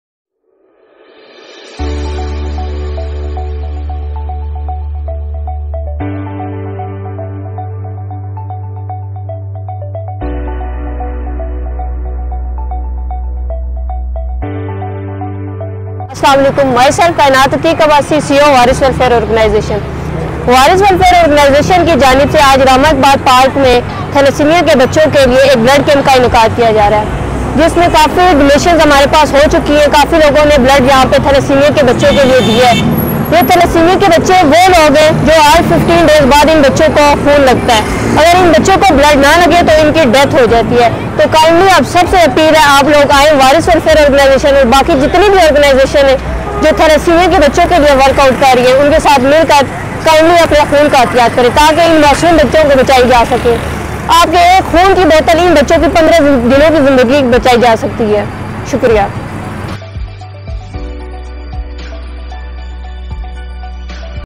Kainat Atiq Abbasi, C.E.O Waris Welfare Talking about Blood Camp. Coverage By Diary TV
D.O.P
Karim Khan

Present by Diary TV Social Media Network
Facebook Ink